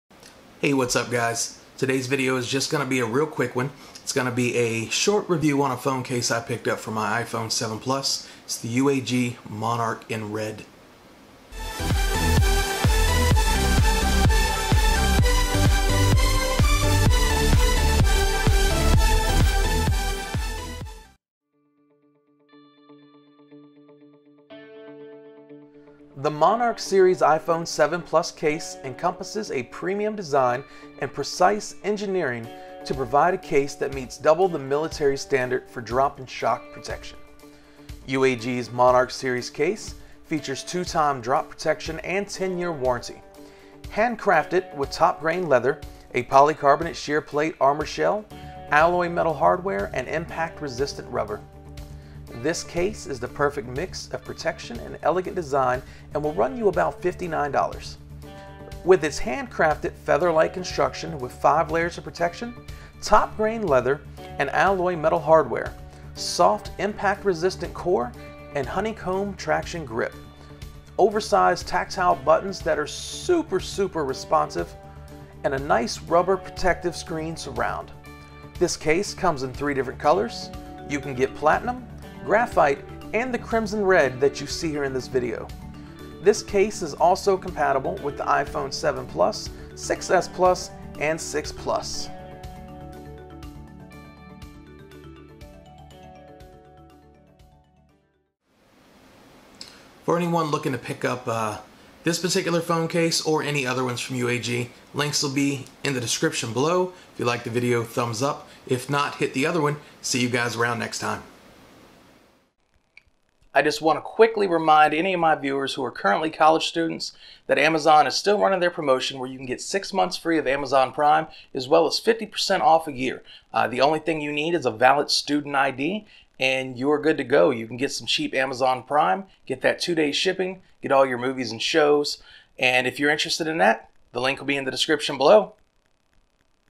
UAG Monarch BEST Rugged iPhone 8 Plus Case
In this video we look at the  UAG Monarch BEST Rugged iPhone 8 Plus Case in Crimson red.